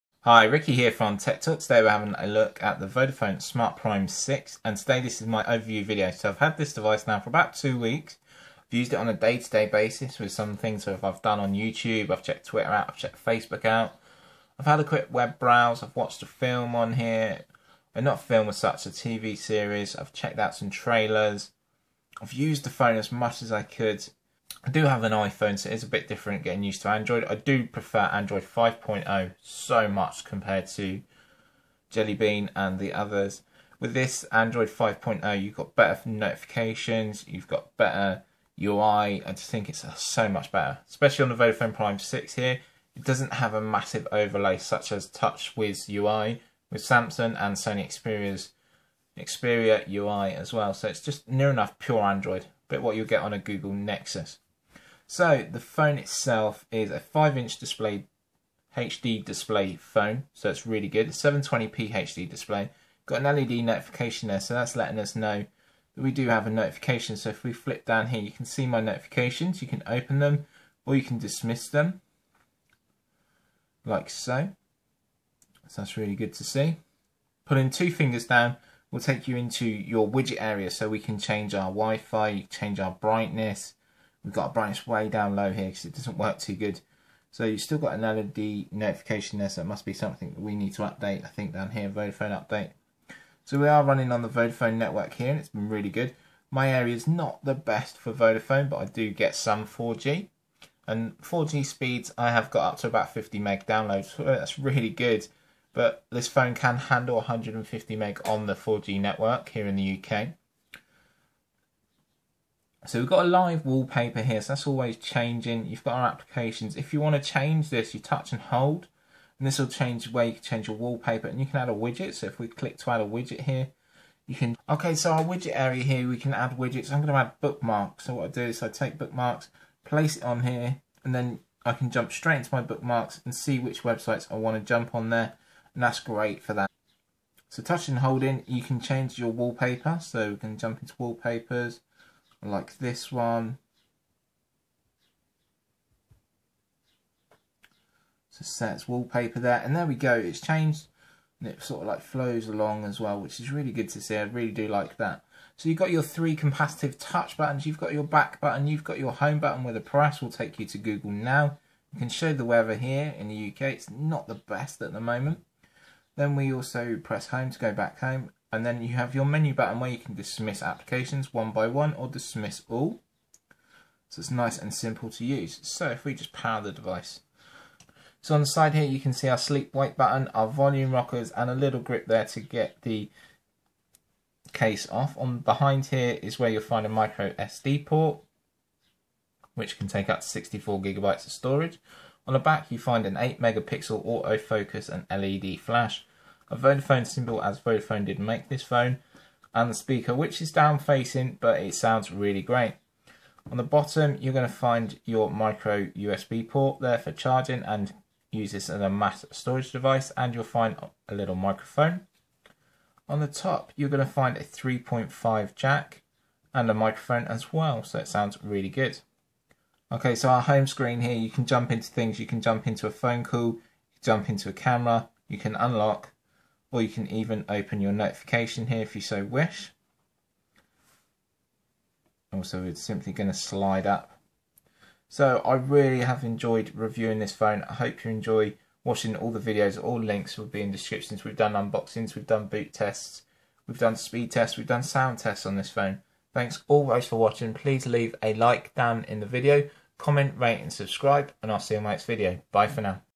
Vodafone Smart Prime 6 Review
In this video which is final one of Vodafone Smart Prime 6 this my review.

Link to buy from Vodafone.